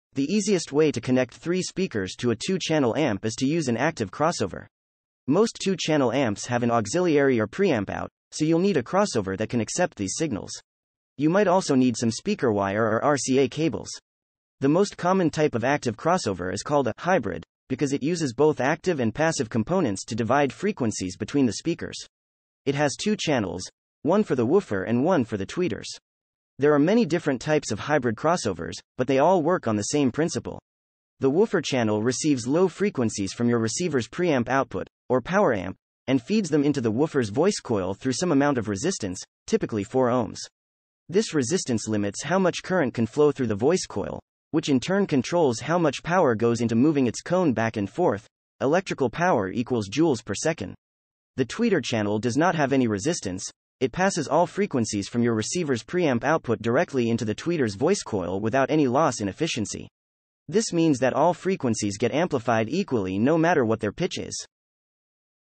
How To Connect 3 Speakers To A 2 Channel Amp?
The easiest way to connect three speakers to a 2-channel amp is to use an active crossover. Most 2-channel amps have an auxiliary or preamp out, so you’ll need a crossover that can accept these signals. You might also need some speaker wire or RCA cables.